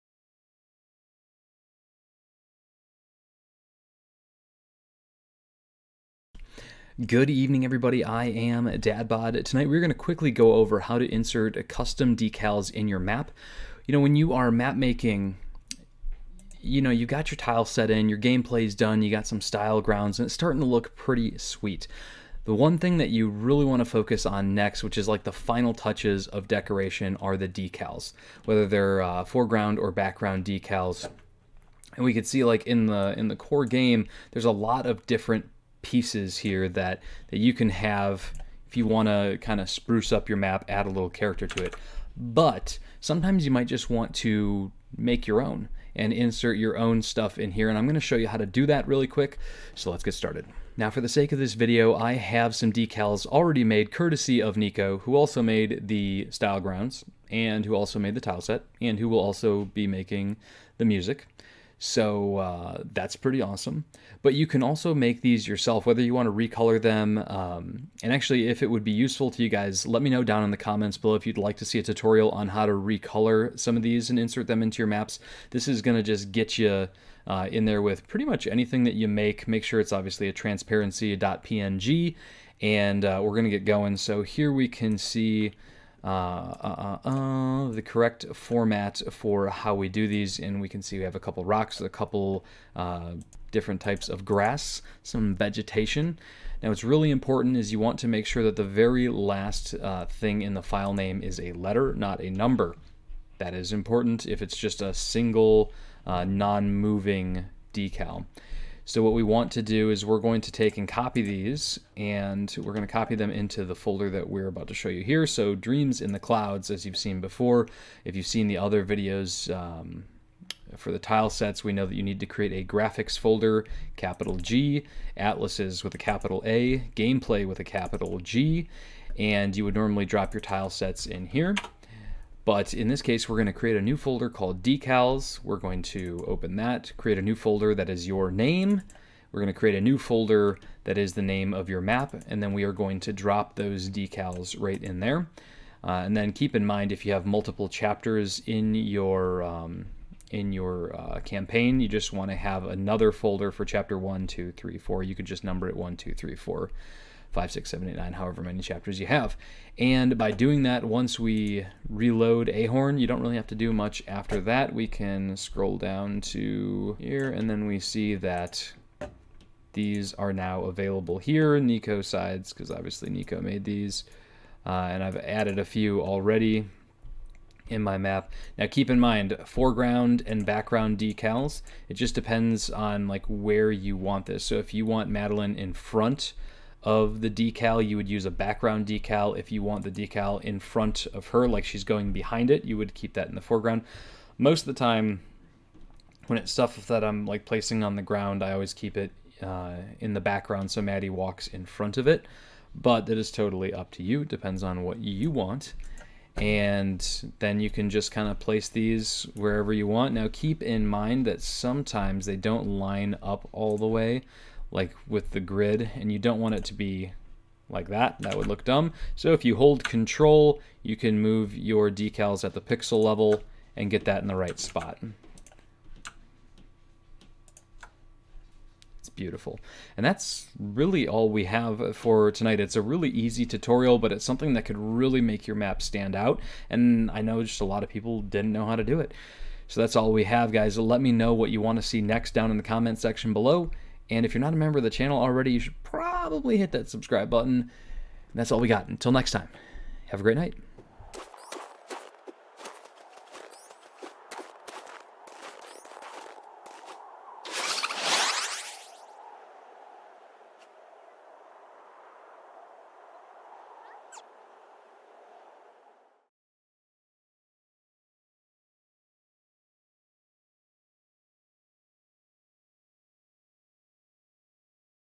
Ahorn Tutorial: Insert Custom Decals
Here we are showing you how to insert your own decals.

Support: Channel Memberships now Available!